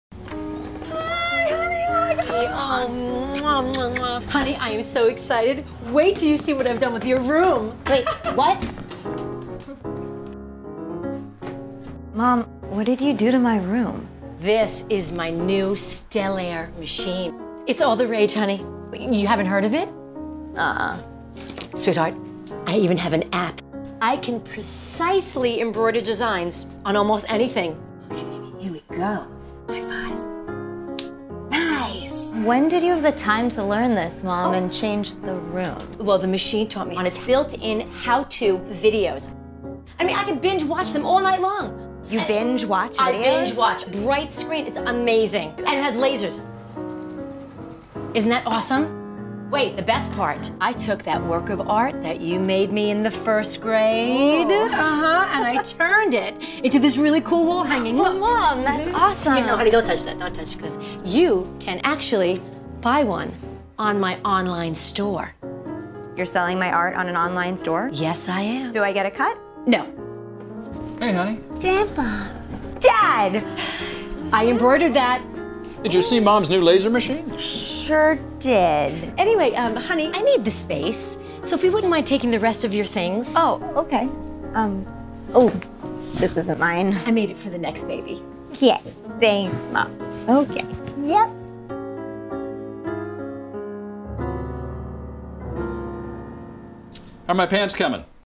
La machine à coudre et à broder Brother Stellaire
Cette machine à coudre et à broder va vous bluffer ! Avec son application mobile My Design Snap, vous allez pouvoir numériser les arrières plan, vos dessins au trait et vos illustrations, et avoir un positionnement précis du motif ! Bref, vous pourrez avoir un résultat très professionnel avec un confort d'utilisation au top.

A bientôt